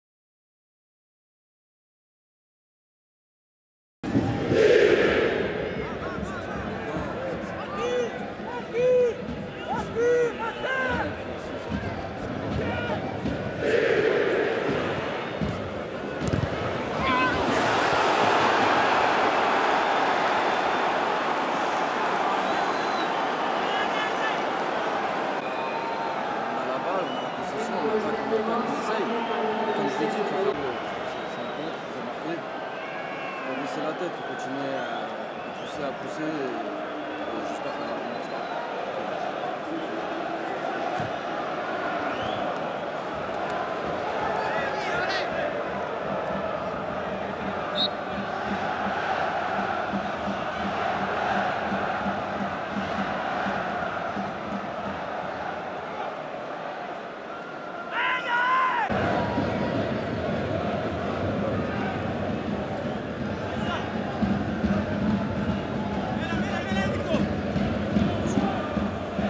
Oldair Fortes scores the SILLIEST goal of the season!!! - Lille VS Reims (1-2) - Ligue 1 - 12/01/14
LOSC Lille vs Stade de Reims (1 - 2) : Odair FORTES (73') goal. All LOSC Lille vs Stade de Reims goals in video.

Stade Pierre Mauroy - Sun, 12 Jan 2014

Goals : Odair FORTES (73' - Stade de Reims) / Grzegorz KRYCHOWIAK (76' - Stade de Reims) / Anthony WEBER (88' csc - LOSC Lille)